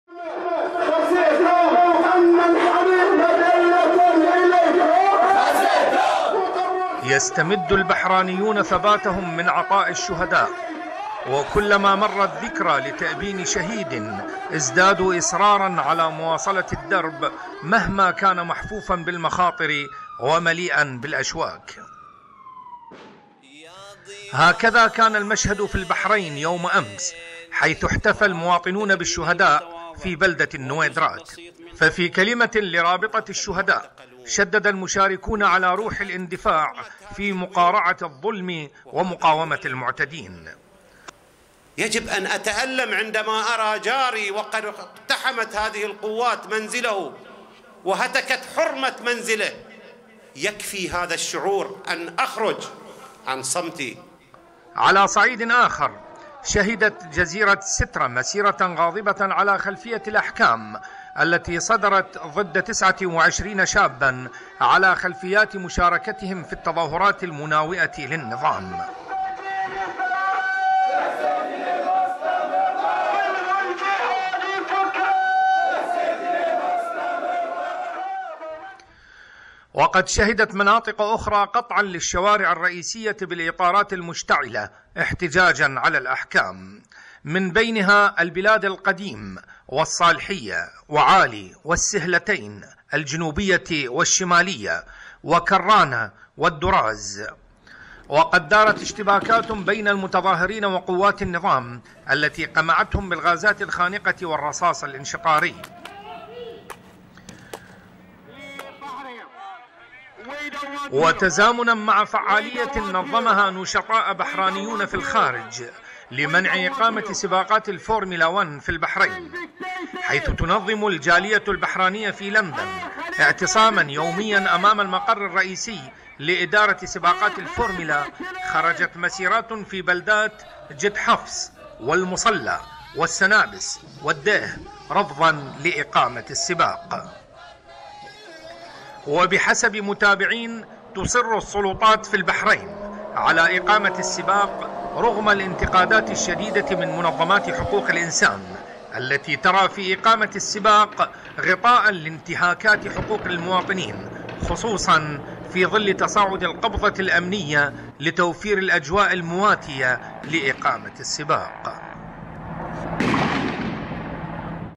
وكالة أنباء البحرين اليوم | 26 مارس 2014 | أحكام "مسيسة" ضد ٢٩ شابا من جزيرة سترة
يستمدُ البحرانيون ثباتهم من عطاءِ الشهداء، وكل ما مرت ذكرى لتأبين ِشهيد ازدادوا  اصرارا على مواصلة الدرب مهما كان محفوفا بالمخاطر ومليئا بالأشواك.

هكذا كان المشهدُ في البحرين يوم َأمس حيث احتفى المواطنون بالشهداء في بلدة النويدرات.
ففي كلمة ٍلرابطة الشهداء شدد المشاركون على روح ِالإندفاع في مقارعة الظلم ومقاومة المعتدين.

على صعيدٍ آخر، شهدت جزيرة سترة مسيرة ًغاضبة ًعلى خلفية الأحكام التي صدرت ضد ٢٩ شابا على خلفيات مشاركتهم في التظاهرات المناوئةِ للنظام.

وقد شهدت مناطق َأخرى قَطعا للشوارع الرئيسية بالإطارات المشتعلة احتجاجا على الأحكام، من بينها البلاد القديم والصالحية وعالي والسِهلتين الجنوبية والشمالية وكرانة والدراز، وقد دارت اشتباكات ٌبين المتظاهرين وقوات النظام التي قمعتهم بالغازات الخانقة والَرصاص الانشطاري.

وتزامنا مع فعاليةٍ نظمها نشطاءَ بحرانيون في الخارج لمنع اقامة سباقات الفورملا1 في البحرين، حيث تنظم الجالية البحرانية في لندن اعتصاما يوميا أمام َالمقر الرئيسي لإدارة سباقات الفورميلا، خرجت مسيرات ٌفي بلدات جدحفص والمصلى والسنابس والديه رفضا لإقامة السباق.

وبحسب متابعين، ُتصرُ السلطات في البحرين على إقامةِ السباق رغم الانتقادات الشديدة من منظمات حقوق الإنسان، التي ترى في اقامة ِالسباق غطاءً لانتهاكات حقوق المواطنين، خصوصا في ظل تصاعد القبضة ِالأمنية لتوفير الأجواء المواتية ِلإقامة السباق..